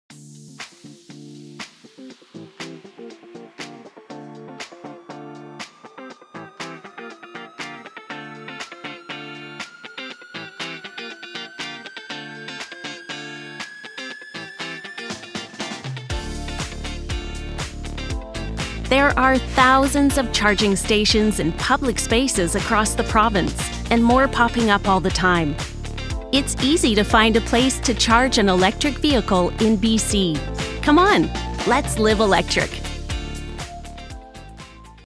Emotive LiveElectric - Charging Stations in BC
Use apps like to find charging stations and learn more about the electric life The public charging network is pretty expansive and constantly growing across British Columbia—including more rural areas. Driving electric has never been easier.

# LiveElectric